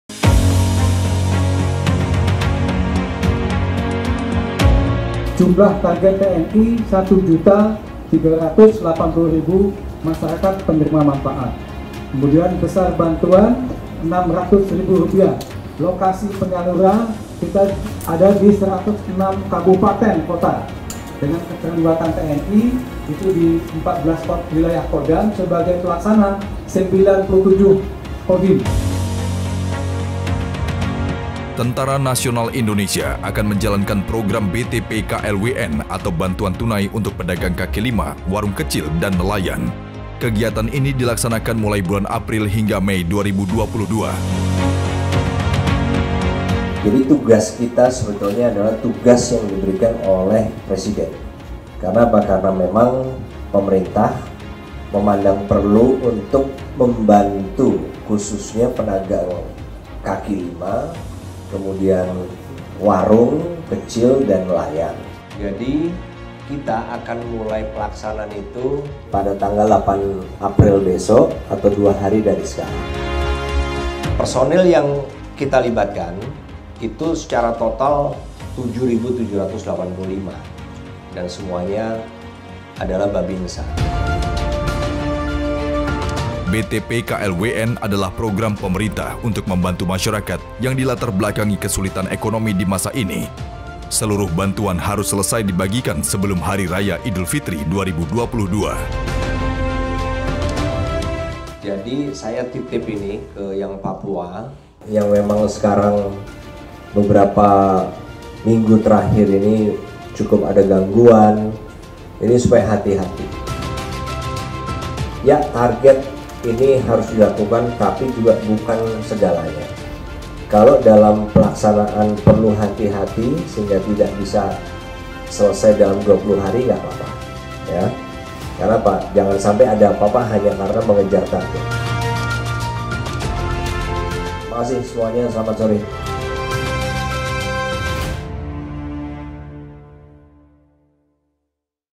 Bantuan Tunai Pedagang Kaki Lima, Warung Kecil dan Nelayan (BTPKLWN) TNI
Tentara Nasional Indonesia akan menjalankan program BTPKLWN atau Bantuan Tunai untuk Pedagang Kaki Lima, Warung Kecil dan Nelayan. Kegiatan ini merupakan program pemerintah untuk membantu masyarakat di tengah masa sulit saat ini.

BTPKLWN TNI akan disalurkan di 106 Kabupaten dan Kota dengan melibatkan 14 wilayah Kodam dan sebagai pelaksana 97 Kodim dengan jumlah 7,785 Babinsa. Ada 1,380,000 masyarakat yang akan menerima bantuan sebesar Rp. 600,000,-.

Andika memerintahkan khusus wilayah Papua dalam melaksanakan harus penuh kehati – hatian. Jika dalam 20 hari belum dapat selesai tidak apa-apa, demi keamanan seluruh prajurit yang bertugas.